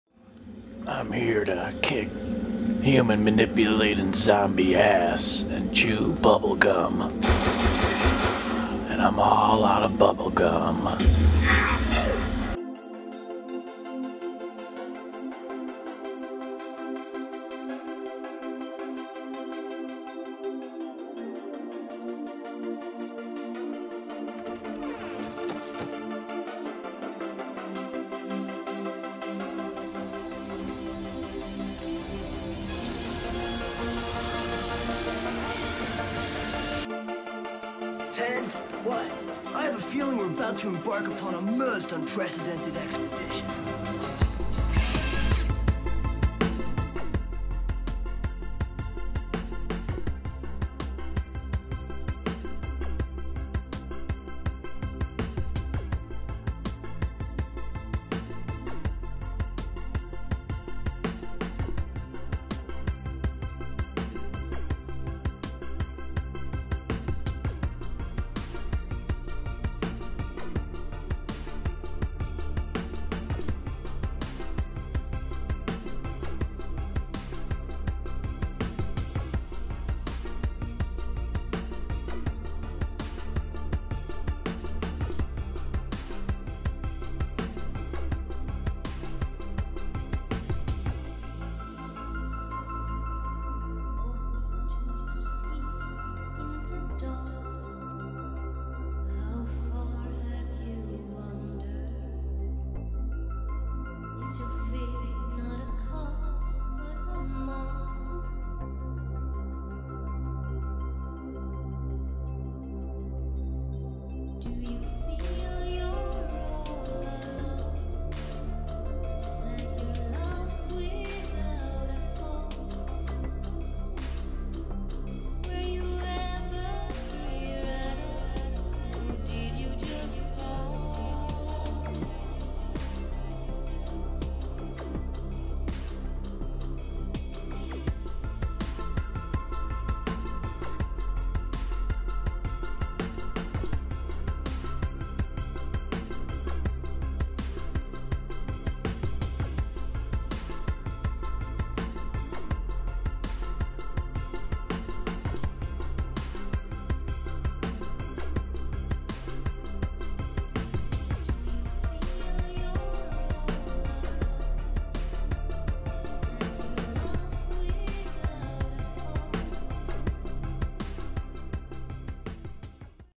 Enter The Purple Zone - He's Dumping What
Past sky observation video clips of interesting craft dispensing things overhead.